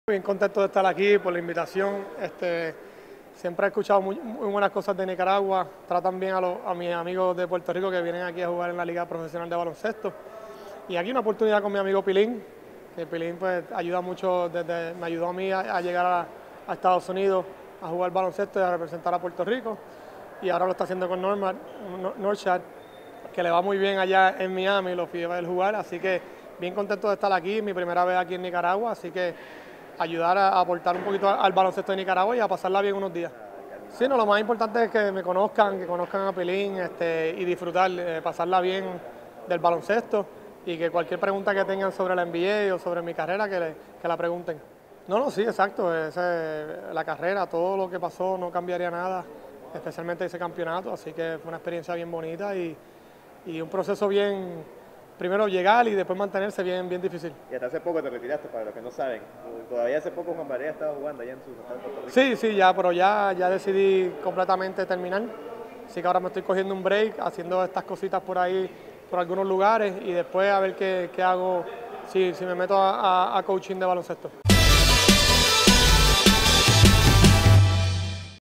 Ex NBA JJ Barea da sus primera palabras al llegar a Nicaragua
Jose Juan Barea jugó 15 temporadas en la NBA y fue campeón una vez con los Mavericks. El boricua está en Managua para dar unas clínicas de baloncesto a jóvenes